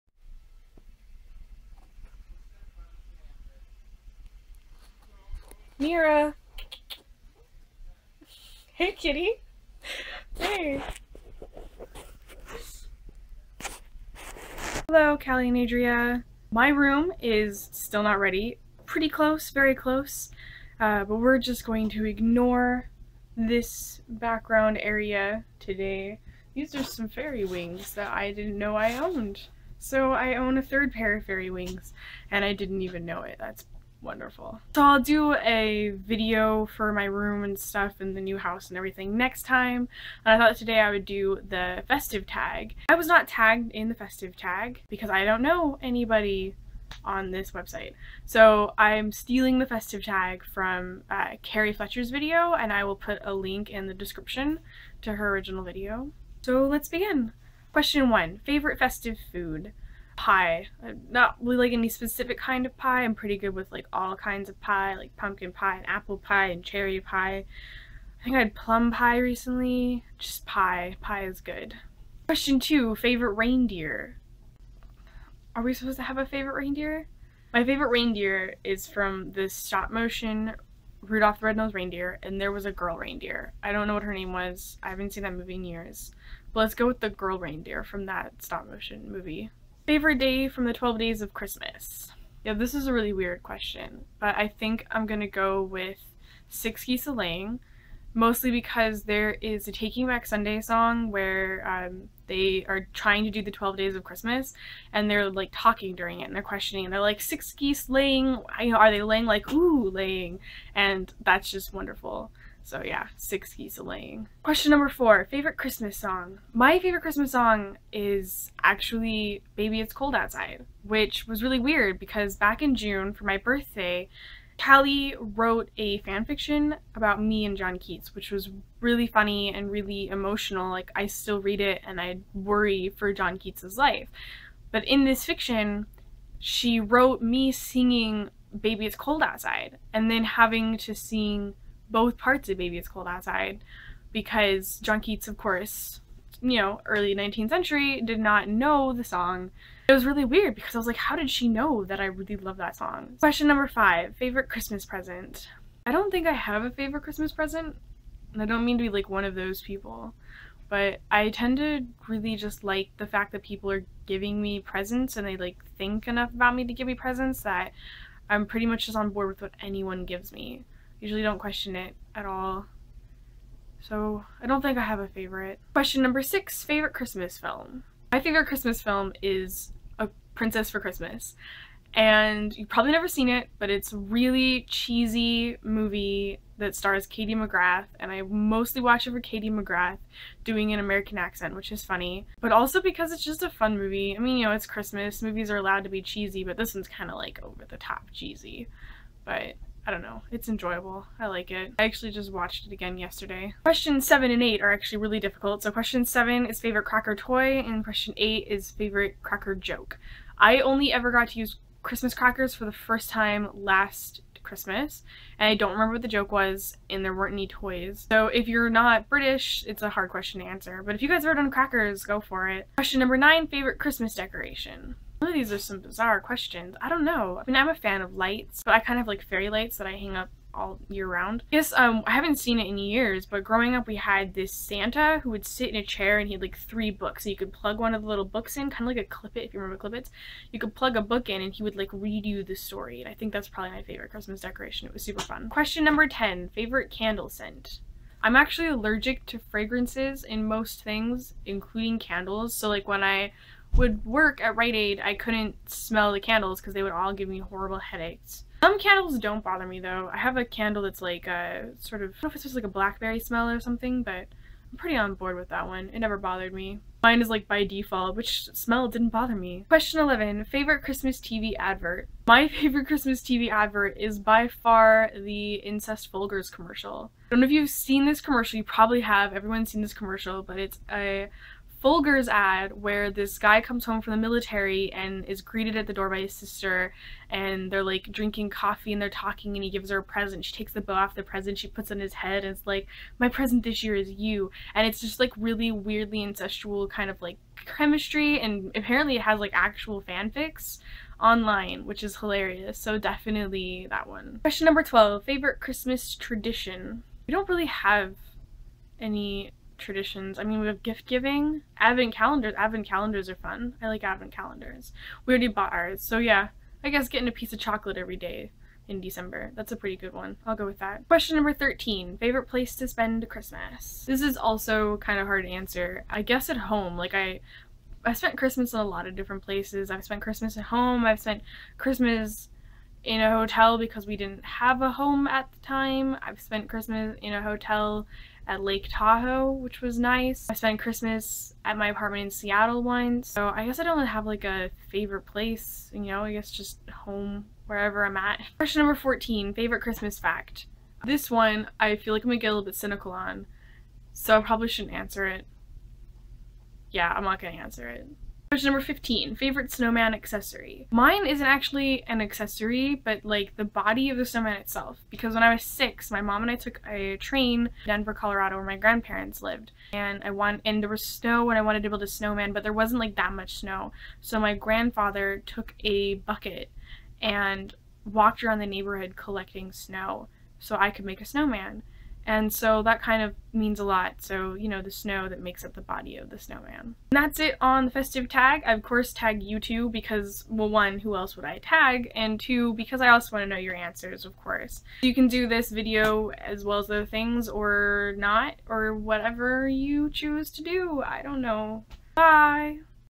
Are We Supposed to Have a Favorite Reindeer? The Festive Tag
Sorry for the terrible quality of the video.
The Questions!
1. Favourite Festive food
2. Favourite Reindeer
3. Favourite day from 12 days of christmas
4. Favourite Christmas Song
5. Favourite Christmas Present
6. Favourite Christmas Film
7. Favourite Cracker Toy
8. Favourite Cracker Joke
9. Favourite Christmas Decoration
10 .Favourite Candle Scent
11. Favourite Christmas TV Advert
12. Favourite Christmas Tradition
13. Favourite Place to Spend Christmas
14. Favourite Christmas Fact
15. Favourite Snowman Accessory